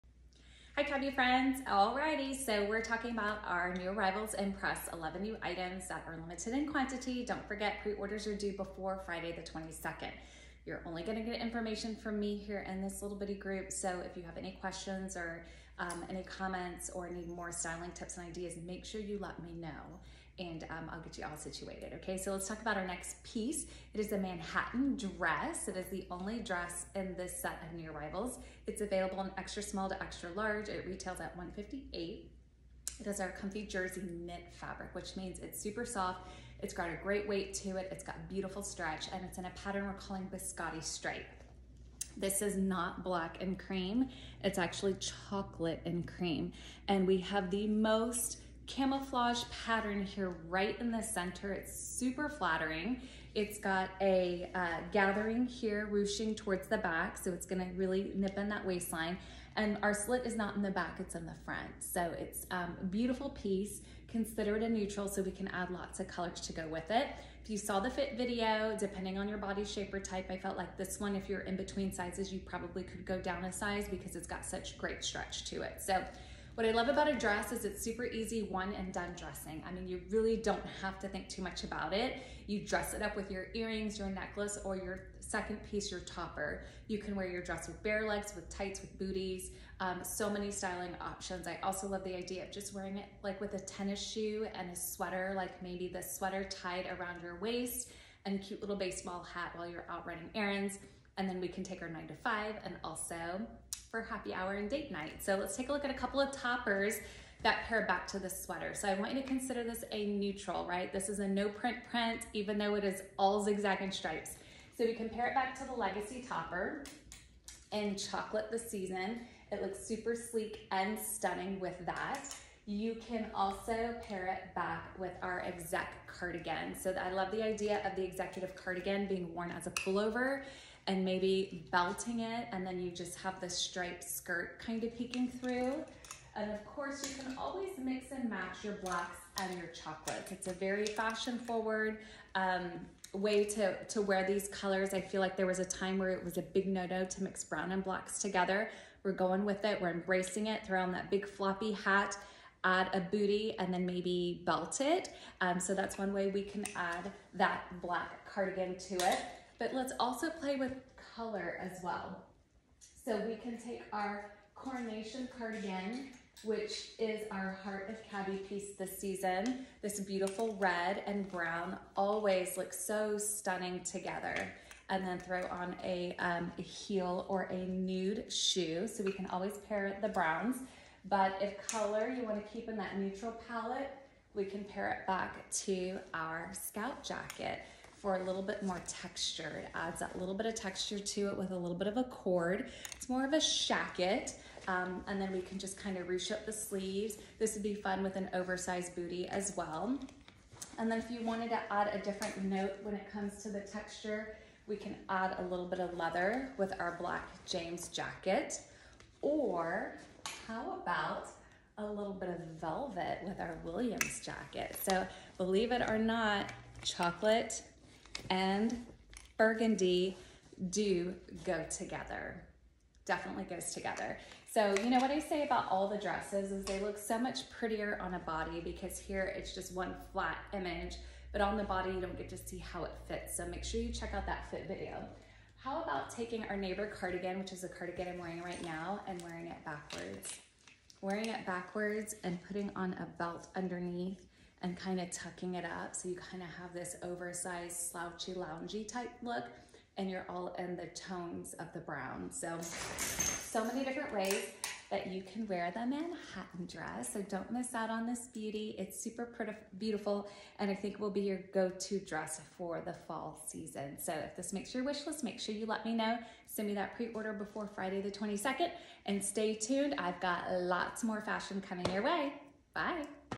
F23 New Arrivals#1 Impress- Manhattan Dress
4614 Manhattan Dress
size: XS-XL
total length: 37 in. | 94 cm.
price: usa 158
content: Self: 94% Rayon, 6% Spandex Lining: 94% Rayon, 6% Spandex
care: Turn garment inside out and place in a mesh bag. Machine wash separately in cold water on the delicate cycle. Only non- chlorine bleach if needed. Do not twist or wring. Lay flat to dry. Warm iron if needed. Can be dry cleaned.
FIT & FEATURES
• Biscotti Stripe • Jersey knit
• Fitted
• V-neckline
• Fully lined bodice
• Pleat detail on the bodice and skirt
• Elastic shirring at the back waist
• Slit in the center front bottom
• 3⁄4 length sleeves
• Hits at the knees
• Our favorite gutsy jersey, like the Spring
’23 Tearoom Dress New Arrival
• Print is dark chocolate and bone; great with the Legacy Topper from the main
Collection
• Waist seam with gathers and tucks along
with the print create flattering tummy camouflage